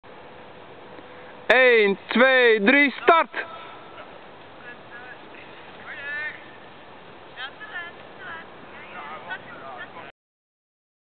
Natuurkunde: Inhalen bij constante snelheid
Een leerling jogt en een andere leerling, met een voorsprong, wandelt met constante snelheid. Daarbij passeren ze andere leerlingen die allemaal op vijf meter afstand van elkaar staan. Maak een tabel met kolommen voor tijd (1x), afstand (2x) en snelheid (2x) en maak één tijd-plaats grafiek met twee lijnen. Bepaal de gemiddelde snelheid van de jogger en van de wandelaar uit de tabel. Bereken de inhaalsnelheid. Bepaal uit de grafiek het inhaaltijdstip.